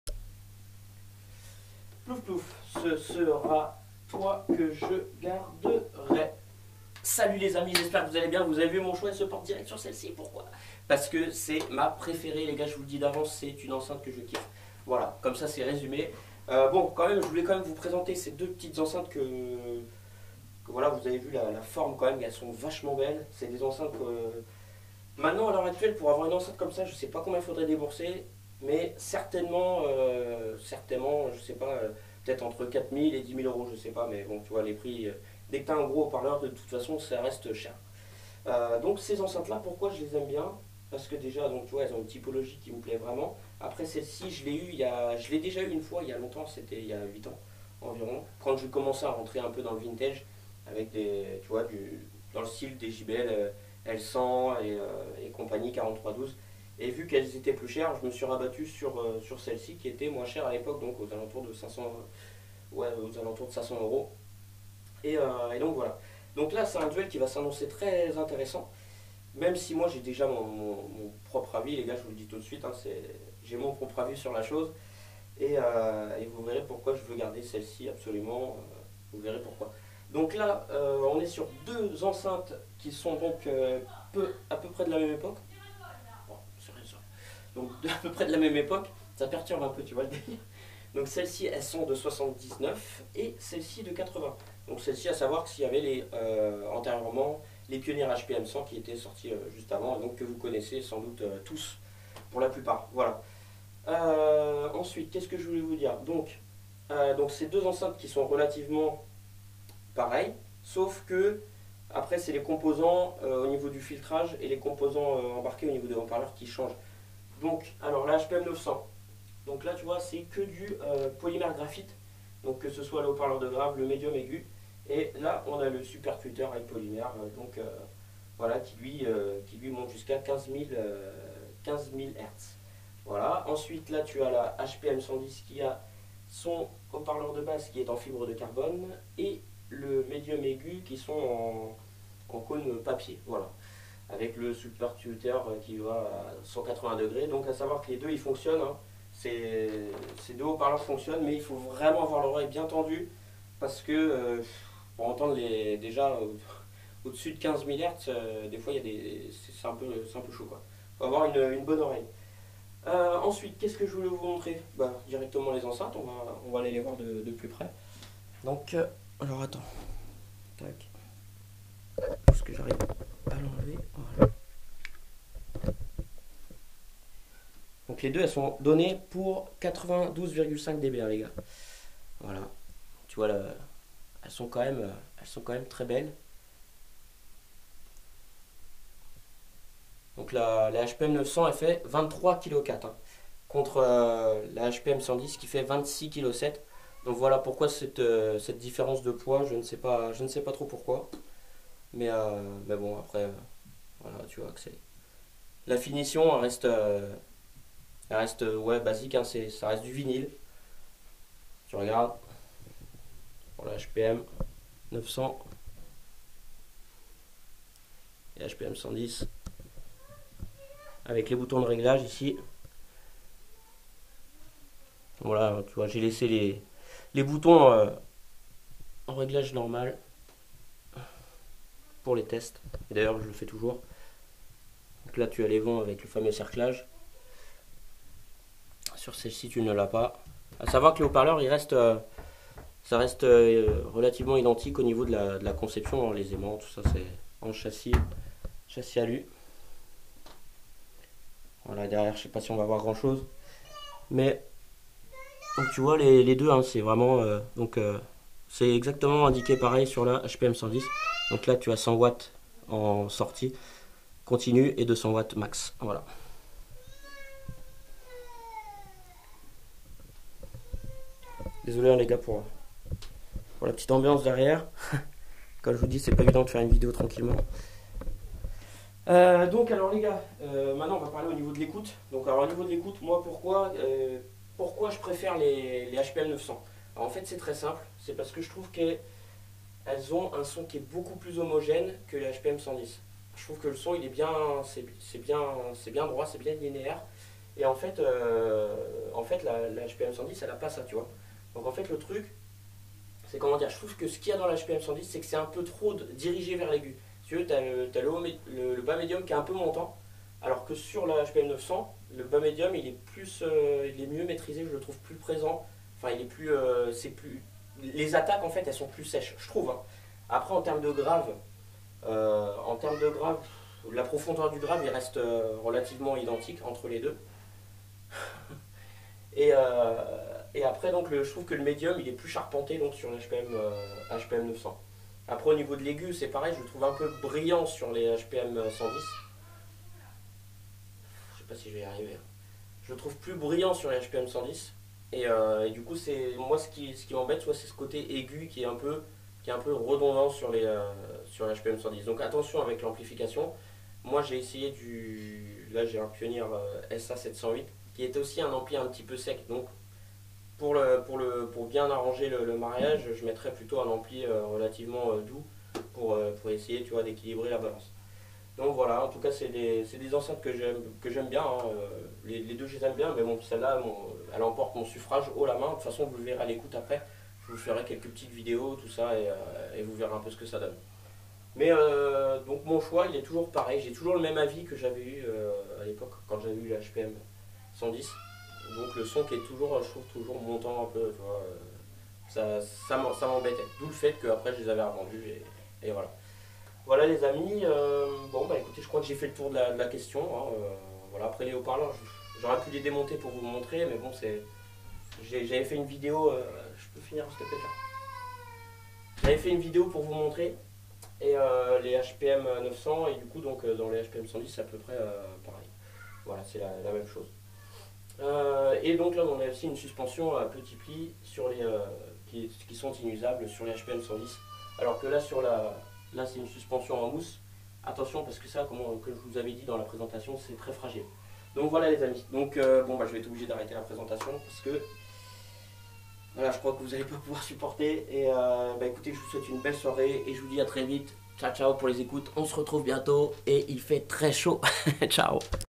Hifi Review Pioneer HPM 900 Vs HPM 110 Presentation Vintage Speakers
petite presentation des hpm900 vs les hpm110 non sans encombre dans cette video que j'ai mis plus d'une heure à faire , c'est la que l'on se rend compte que vie de famille et partager sa passion sont des choses à ne pas melanger, voilà pourquoi j'espere que vous vous rendez compte que meme si je fait des videos à la hate c'est vraiment pour le partage de cette passion commune qui nous anime tous